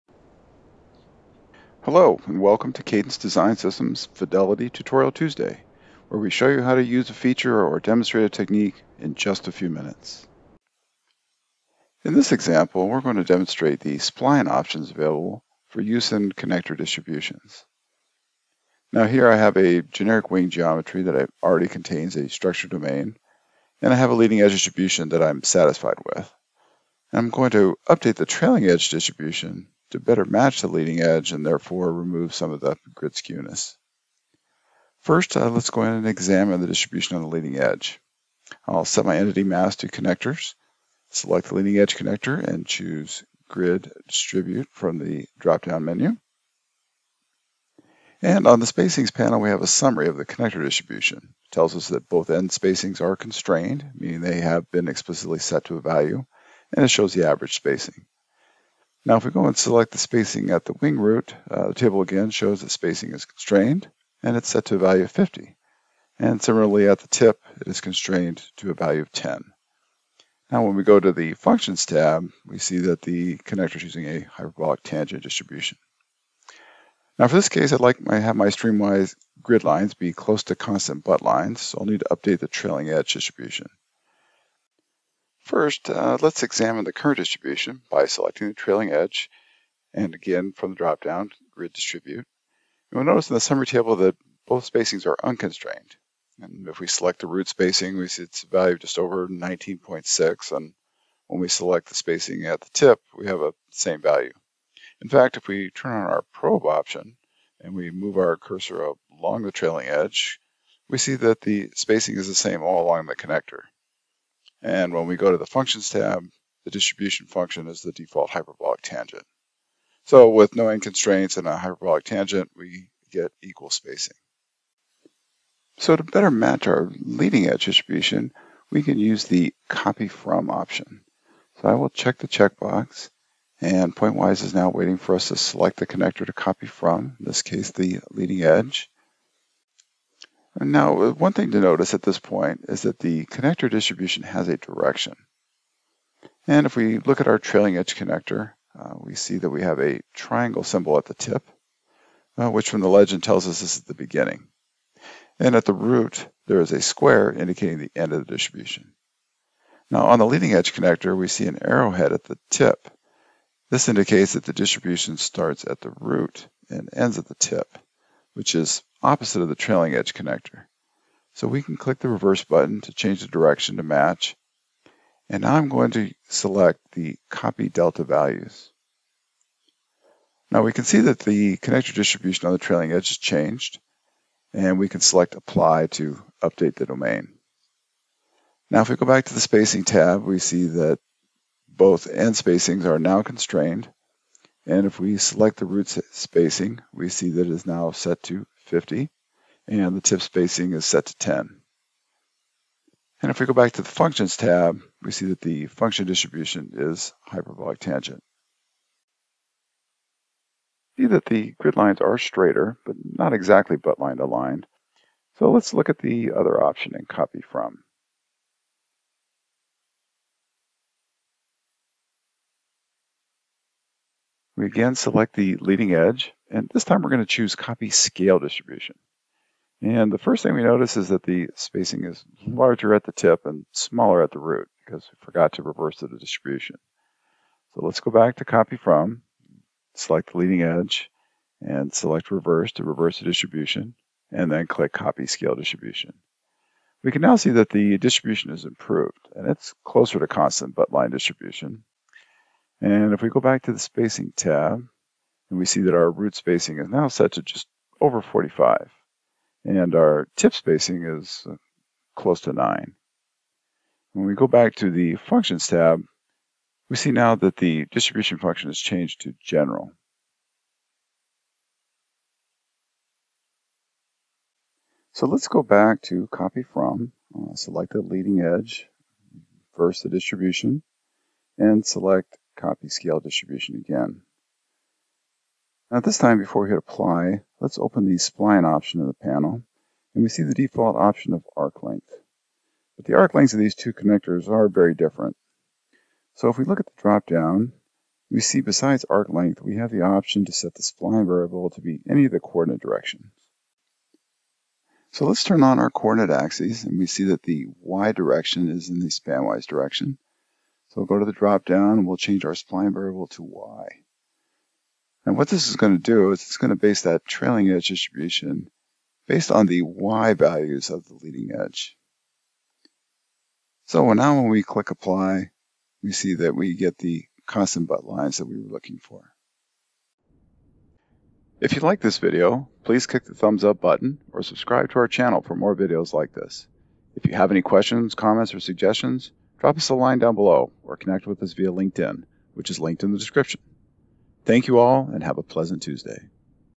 Fidelity Pointwise: Connector Distribution Spline Options
Learn how to change the spline options in the connector distribution to improve the quality of your structured domains in Fidelity Pointwise.